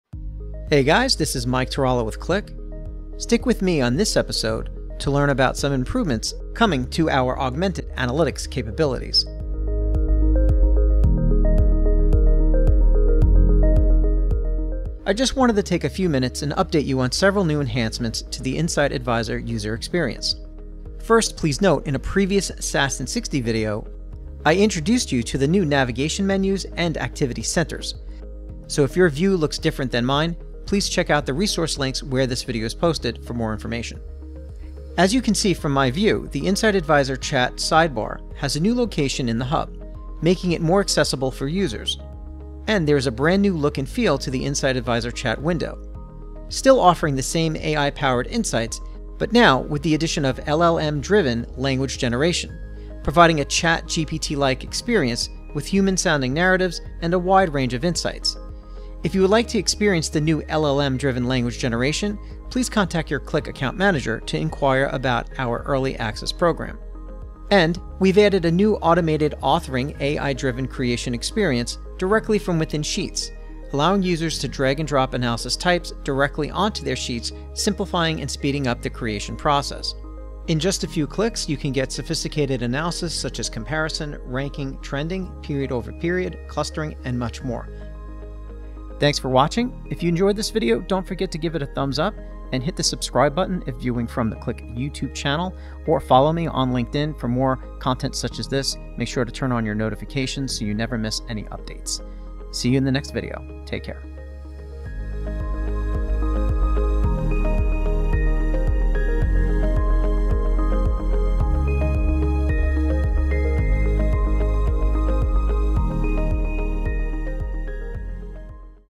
Coming Soon - New Augmented Analytics Enhancements - Do More with Qlik
Coming Soon - The Insight Advisor Chat Sidebar has a new location in the hub, making it more accessible for users, and there is a brand new look and feel to the Insight Advisor chat window,  still offering  the same AI-powered insights but now with the addition of LLM-driven language generation, providing a "Chat GPT like" experience with human-sounding narratives and a wide range of insights. If you would like to experience this new LLM-driven language generation, please contact your Qlik Account manager to inquire about our Early Access Program. Secondly we added a new automated authoring AI-driven creation experience directly from within sheets, allowing users to drag and drop analysis types directly onto their sheets, simplifying and speeding up the creation process.